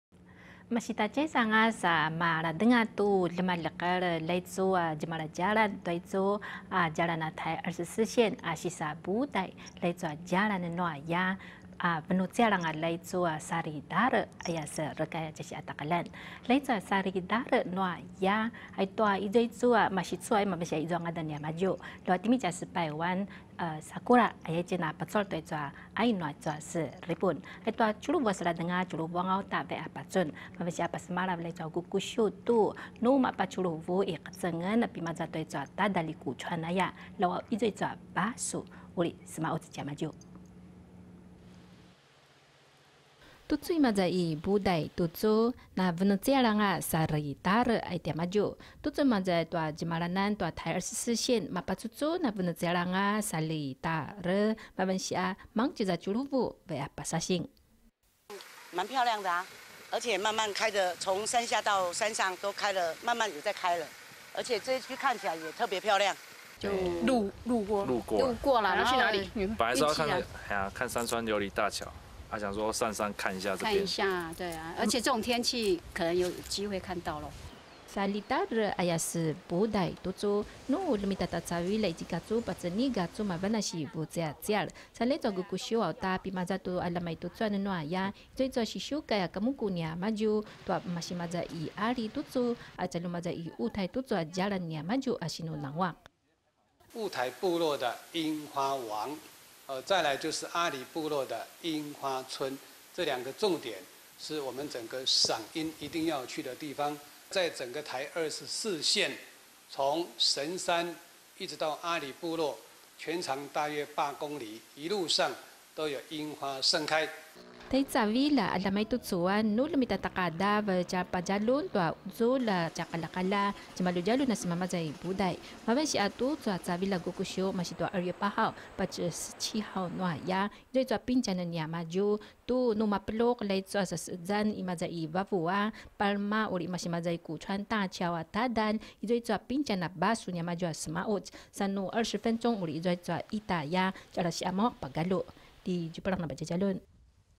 阿禮部落櫻花開 接駁公車解車潮 2016-02-01 Paiwan TITV 原視族語新聞
uri izuwa basu a sema'uc a sema Buday

在一波波寒流接連襲台後，台24線霧台鄉鄰近的阿禮部落路段櫻花已經開始綻放，也有遊客開始上山「追櫻」。今年霧台鄉公所，在春節連續假期期間，首次提供接駁公車，希望疏導龐大車潮。

屏東霧台鄉櫻花，花期不受最近不穩定的氣候影響，目前進入盛開期，台24線沿路兩旁，可見粉嫩櫻花，吸引遊客賞櫻，拿出手機拍下美景。

霧台鄉櫻花屬於台灣原生種，每年1月底到2月，是盛開期，霧台鄉公所年節期間，也將進行櫻花季活動，霧台部落櫻花王跟阿禮部落的櫻花村，是賞櫻最佳景點。

104年櫻花季，霧台每日都吸引上千遊客前來賞櫻，今年鄉公所2月8日到17日，將首次管制車輛，只要部落停車位滿，就會在谷川大橋處啟動接駁公車，每20分鐘會有一班，可望緩解進入部落車潮。